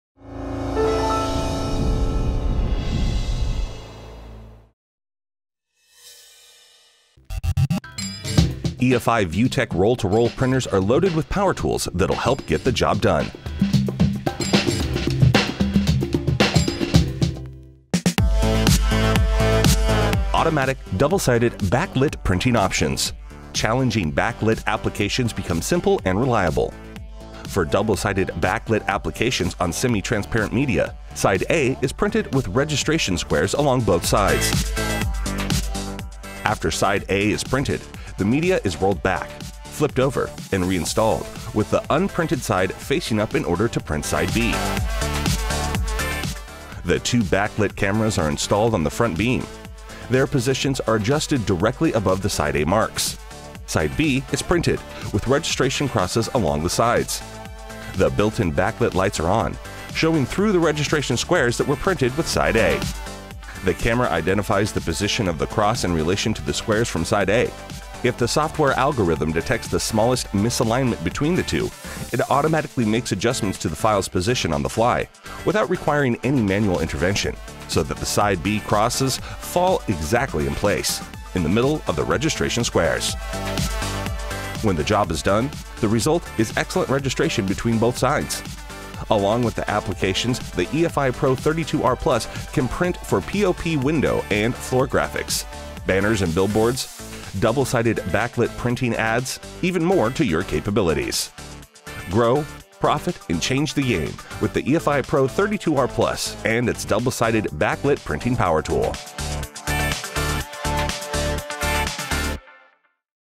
EFI Pro 32r+ Automatic Backlit Power Tool
With the automatic double-sided backlit printing option, challenging backlit applications become simple and reliable. Camera-guided software automatically corrects registration on the x, y and tilt axes, eliminating the need for guesswork and manual manipulation. The result is perfect registration on both sides.